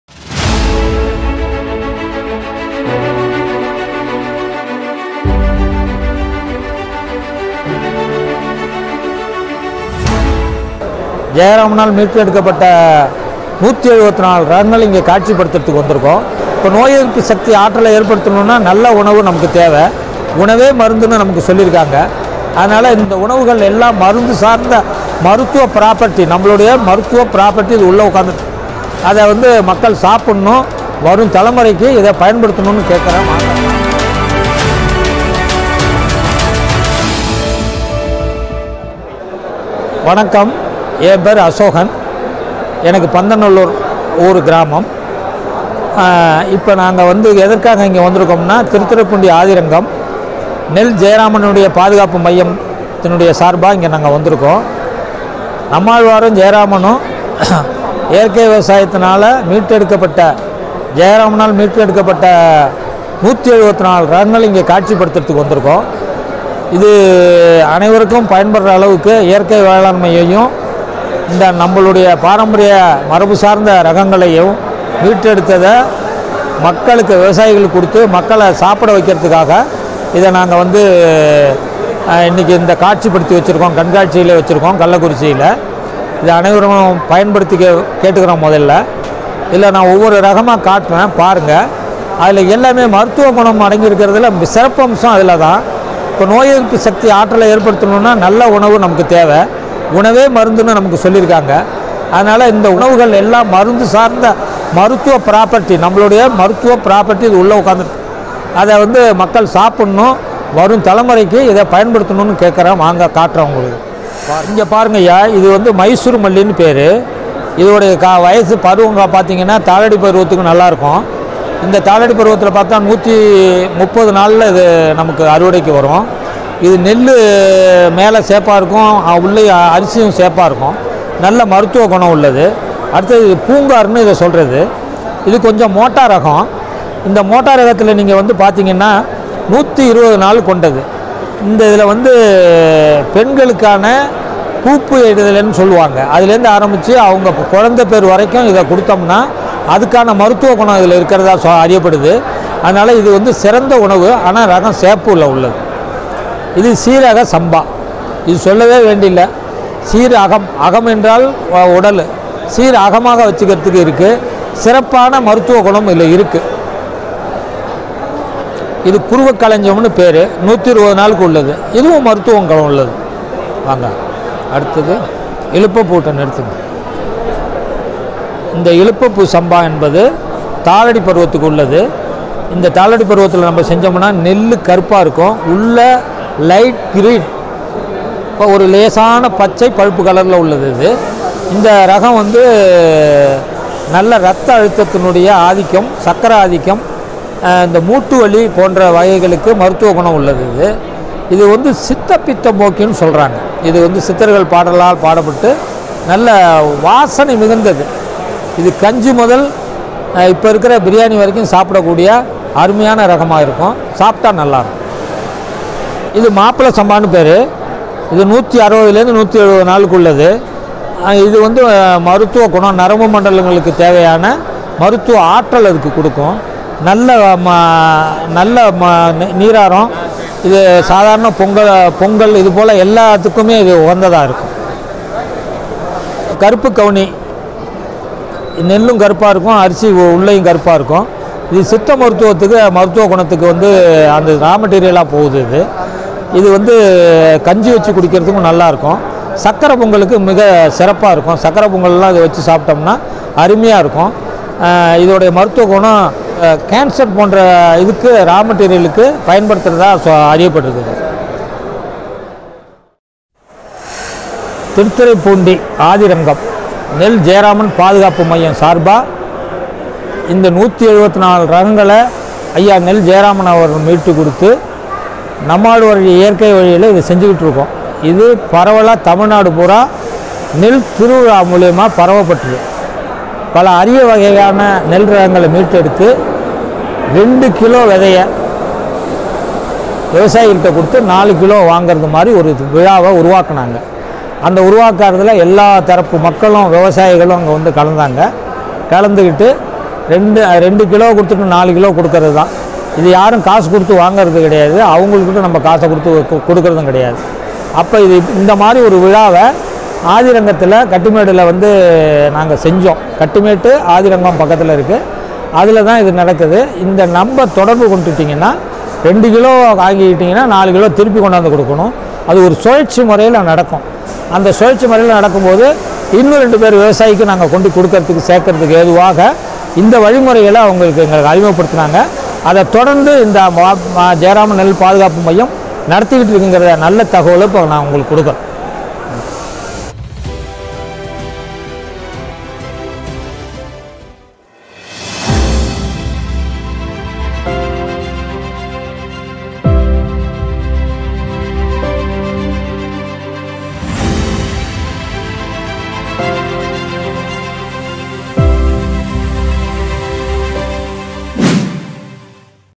தமிழகத்தின் பாரம்பரிய 174 நெல் இரகங்கள் || நெல் ஜெயராமன் பாரம்பரிய நெல் பாதுகாப்பு மையம்
தமிழகத்தின் பாரம்பரிய 174 நெல் இரகங்கள் || நெல் ஜெயராமன் பாரம்பரிய நெல் பாதுகாப்பு மையம் || நோய் எதிர்ப்பு சக்தி உருவாக்கும் நெல் இரகங்கள்?||

நெல் ஜெயராமன் பாரம்பரிய நெல் பாதுகாப்பு மையம்,
ஆதிரங்கம், திருத்துறைப்பூண்டி வட்டம்,